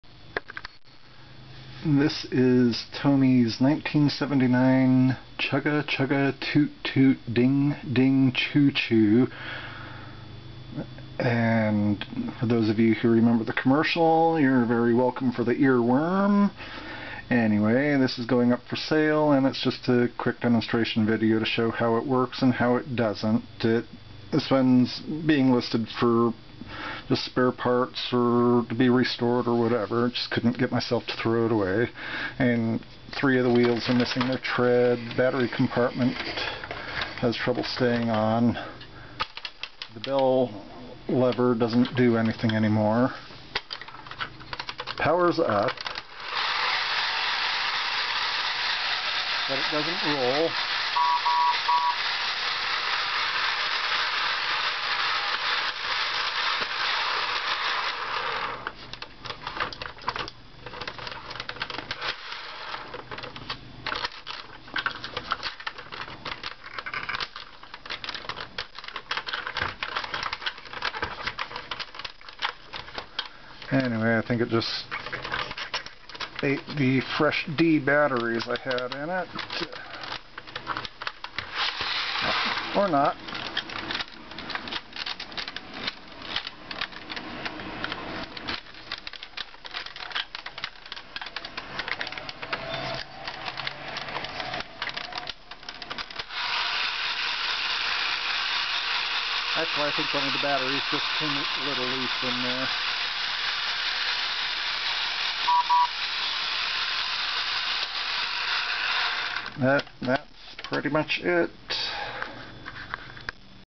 Sale Item Demo - Chugga Chugga Toot Toot Ding Ding Choo Choo - Partially Working - Sold on Etsy
Quick demo for a partially working Tomy Chugga Chugga Toot Toot Ding Ding Choo Choo that is going up for sale.

Sold on Etsy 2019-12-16 for $9.95